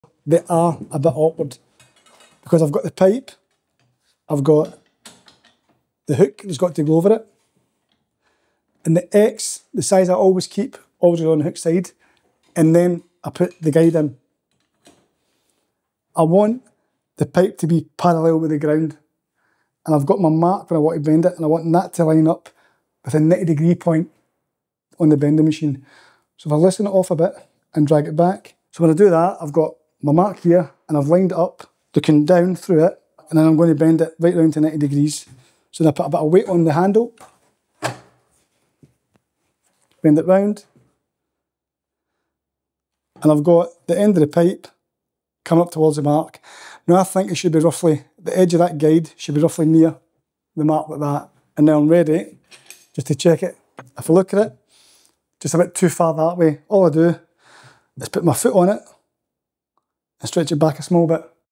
👉 Plumbing Tip: Why Keeping the Pipe Parallel Matters When Bending a 90°
This video shows a free-hand method for bending a clean 90° in copper pipe, commonly used in plumbing when a pipe bender isn’t mounted in a vice.

The focus here is on pipe control rather than force — keeping the pipe parallel as the bend starts, spotting twist early, and making small corrections to keep the bend accurate and square. This is especially important when forming bends that need to line up cleanly for valves, offsets, or visible pipework.

This technique is regularly used on site and is useful for apprentices and plumbers looking to improve consistency and accuracy when bending copper pipe by hand.

TOOLS USED
Hilmor Pipe Bender
Pipe Cutters
Adjustable Wrench
Knipex Cobra Pliers
Monument Torch
Rothenberger Torch
Bahco Wrench